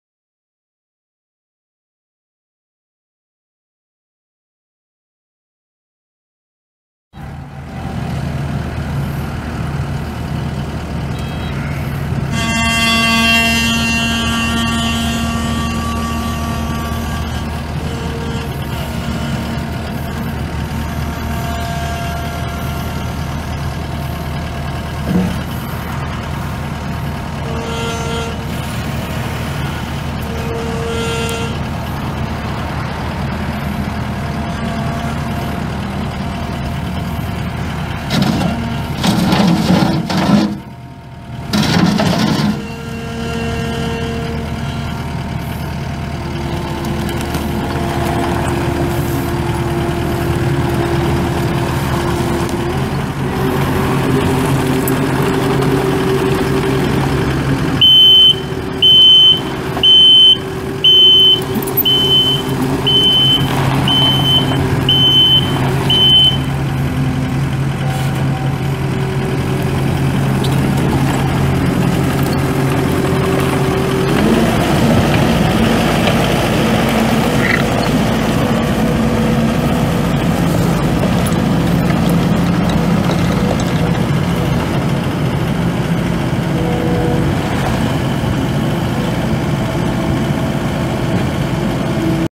2015 Kubota SVL75-2 skid steer for sale at auction | bidding closes June 12, 2018
2015 Kubota SVL75-2 skid steer
701 hours on meter
Kubota V3307-CR four cylinder turbo diesel engine
Two speed hydrostatic transmission
OROPS
Joystick controls
EMS panel
Suspension seat
Front and rear work lights
Auxiliary hydraulics
Manual quick coupler
Kubota AP-SL74SLL bucket
74"W
12"W tracks
Serial 24609
It's located in Davis, OK, and we're selling it to the highest bidder regardless of price.